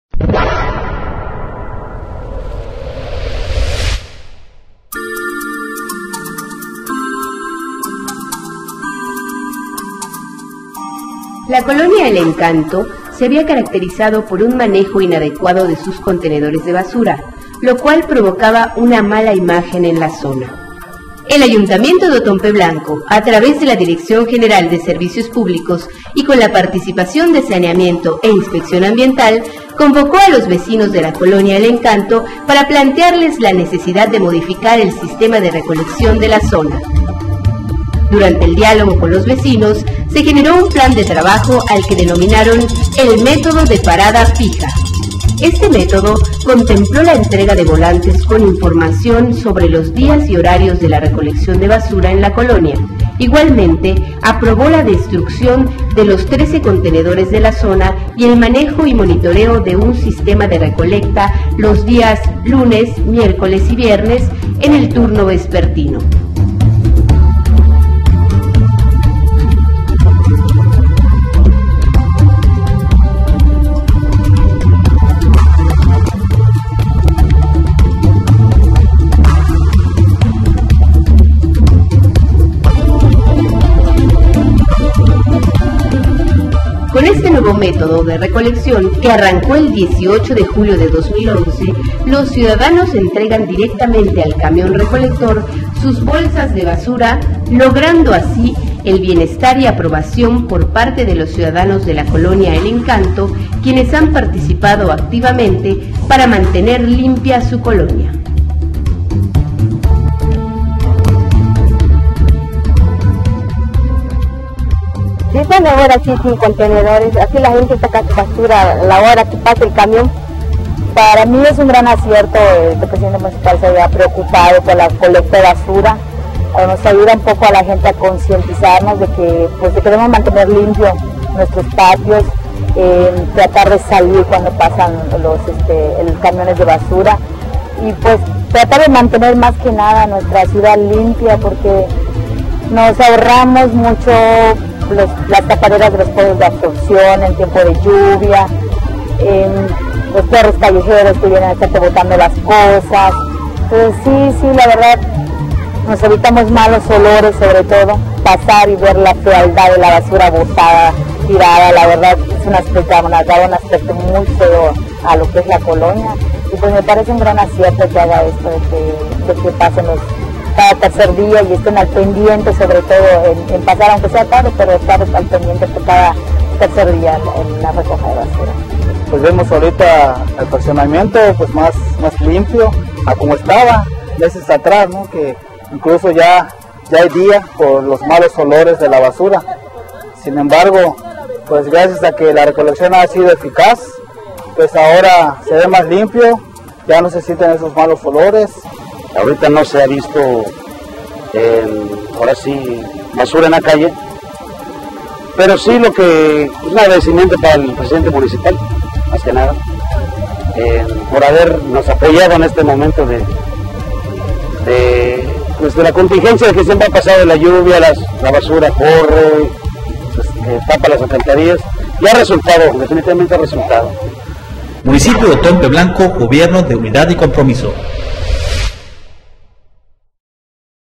EL ENCANTO
Es un video sobre la implementación de acciones que realiza el Municipio de Othón P. Blanco en materia de limpieza en la colonia El Encanto de la ciudad de Chetumal.